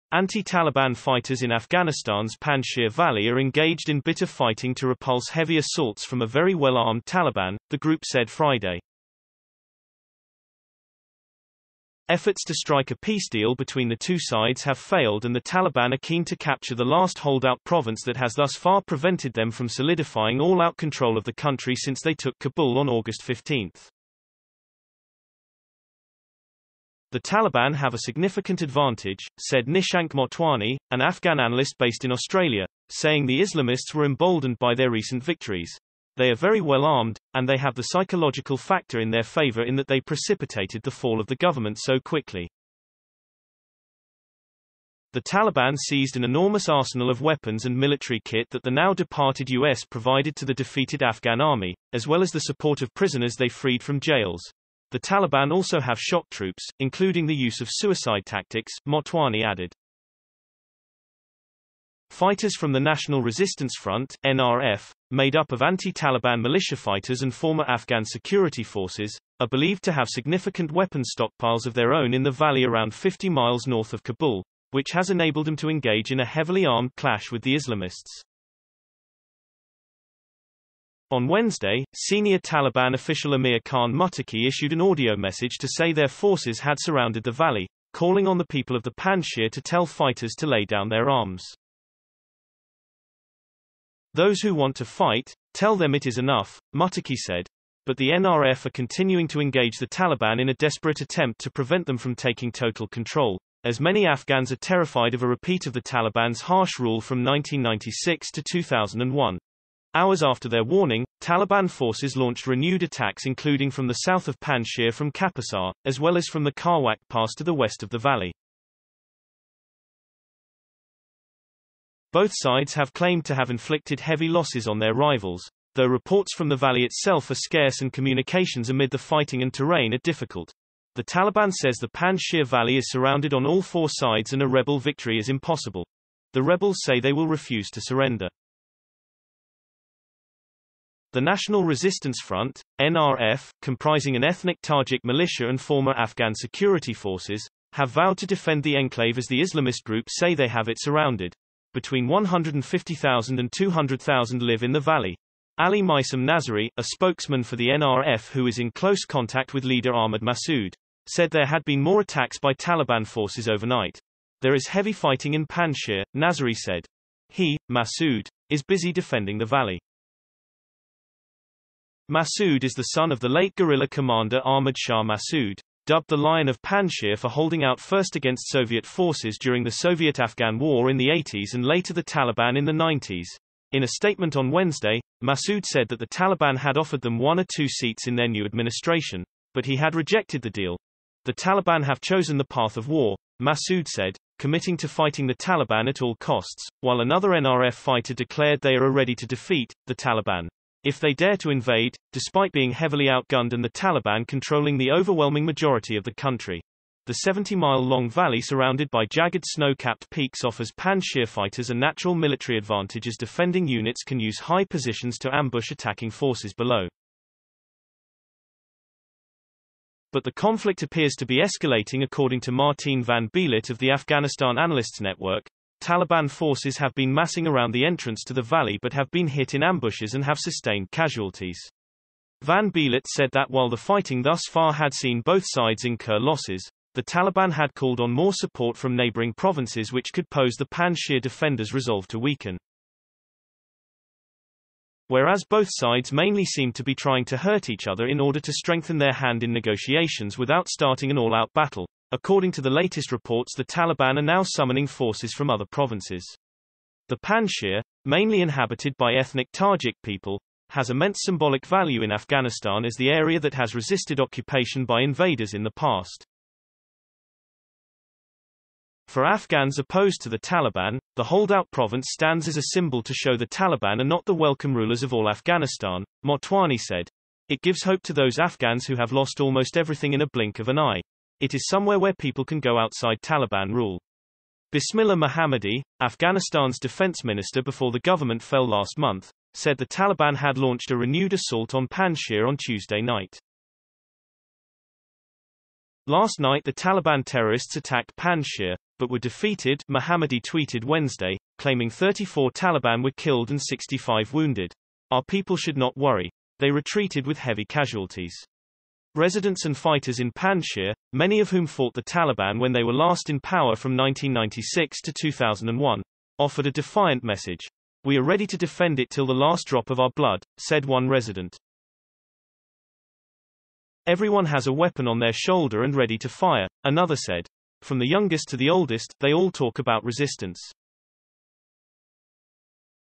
Afghan resistance fighters come under 'heavy assaults' from 'very well armed' Taliban forces
Anti-Taliban fighters in Afghanistan's Panjshir Valley are engaged in bitter fighting to repulse 'heavy' assaults from a 'very well armed' Taliban, the group said Friday..Efforts to strike a peace deal between the two sides have failed and the Taliban are keen to capture the last holdout province that has thus far prevented them from solidifying all-out control of the country since they took Kabul on August 15..'The Taliban have a significant advantage,' said Nishank Motwani, an Afghan analyst based in Australia, saying the Islamists were emboldened by their recent victories.'They are very well armed, and they have the psychological factor in their favour in that they precipitated the fall of the government so quickly.'.The Taliban seized an enormous arsenal of weapons and military kit that the now departed US provided to the defeated Afghan army, as well as the support of prisoners they freed from jails.'The Taliban also have shock troops, including the use of suicide tactics,' Motwani added..Fighters from the National Resistance Front (NRF), made up of anti-Taliban militia fighters and former Afghan security forces, are believed to have significant weapon stockpiles of their own in the valley around 50 miles north of Kabul, which has enabled them to engage in a heavily armed clash with the Islamists..On Wednesday, senior Taliban official Amir Khan Muttaqi issued an audio message to say their forces had surrounded the valley, calling on the people of the Panjshir to tell fighters to lay down their arms.'.Those who want to fight, tell them it is enough,' Muttaqi said.But the NRF are continuing to engage the Taliban in a desperate attempt to prevent them from taking total control, as many Afghans are terrified of a repeat of the Taliban's harsh rule from 1996 to 2001.Hours after their warning, Taliban forces launched renewed attacks including from the south of Panjshir from Kapisa, as well as from the Khawak pass to the west of the valley..Both sides have claimed to have inflicted heavy losses on their rivals, though reports from the valley itself are scarce and communications amid the fighting and terrain are difficult.The Taliban says the Panjshir valley is surrounded on all four sides and a rebel victory is impossible.The rebels say they will refuse to surrender..The National Resistance Front (NRF), comprising an ethnic Tajik militia and former Afghan security forces, have vowed to defend the enclave as the Islamist group say they have it surrounded.Between 150,000 and 200,000 live in the valley.Ali Maisam Nazary, a spokesman for the NRF who is in close contact with leader Ahmad Massoud, said there had been more attacks by Taliban forces overnight.'There is heavy fighting in Panjshir,' Nazary said.'He (Massoud) is busy defending the valley.'.Massoud is the son of the late guerrilla commander Ahmad Shah Massoud, dubbed the 'Lion of Panjshir' for holding out first against Soviet forces during the Soviet-Afghan War in the 80s and later the Taliban in the 90s.In a statement on Wednesday, Massoud said that the Taliban had offered them 'one or two seats' in their new administration, but he had rejected the deal.'The Taliban have chosen the path of war,' Massoud said, committing to fighting the Taliban at all costs, while another NRF fighter declared they 'are are ready to defeat [the Taliban] if they dare to invade,' despite being heavily outgunned and the Taliban controlling the overwhelming majority of the country.The 70-mile-long valley surrounded by jagged snow-capped peaks offers Panjshir fighters a natural military advantage as defending units can use high positions to ambush attacking forces below..But the conflict appears to be escalating according to Martine van Bijlert of the Afghanistan Analysts Network: 'Taliban forces have been massing around the entrance to the valley but have been hit in ambushes and have sustained casualties.' Van Bijlert said that while the fighting thus far had seen both sides incur losses, the Taliban had called on more support from neighbouring provinces which could pose the Panjshir defenders' resolve to weaken.'.Whereas both sides mainly seemed to be trying to hurt each other in order to strengthen their hand in negotiations without starting an all-out battle, according to the latest reports the Taliban are now summoning forces from other provinces.'